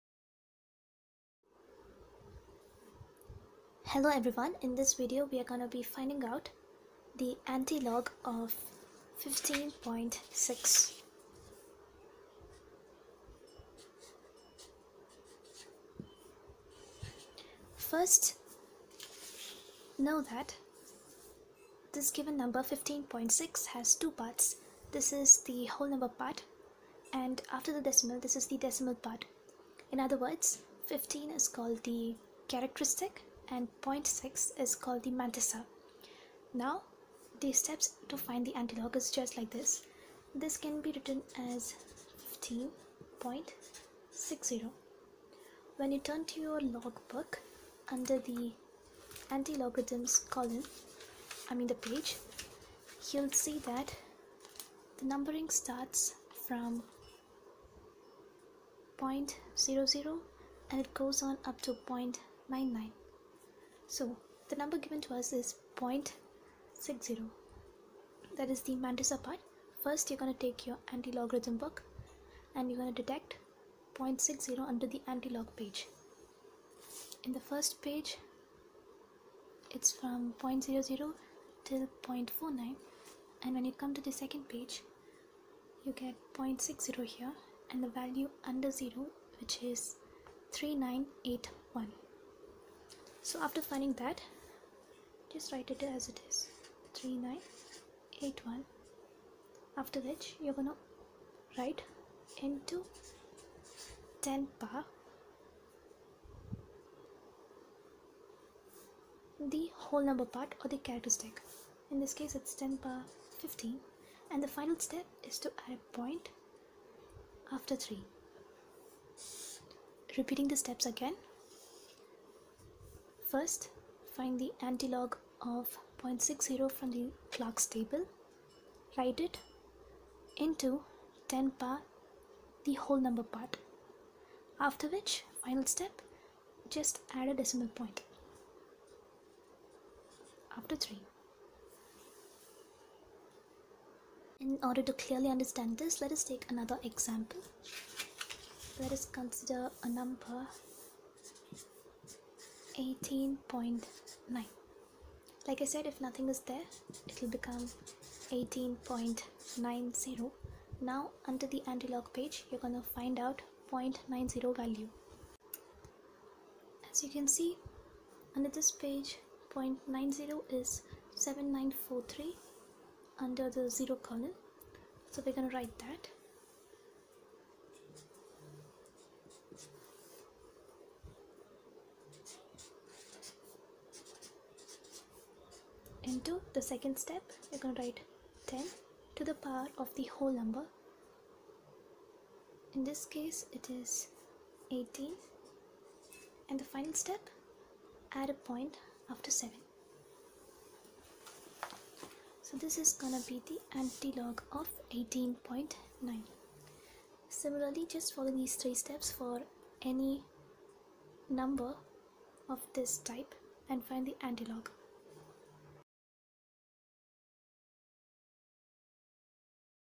How to find Antilog of 15.6 | NCERT Electrochem Worked Example
Calculate the equilibrium constant for the reaction. Eg 3.2 electrochemistry chapter NCERT Class 12 and Second PUC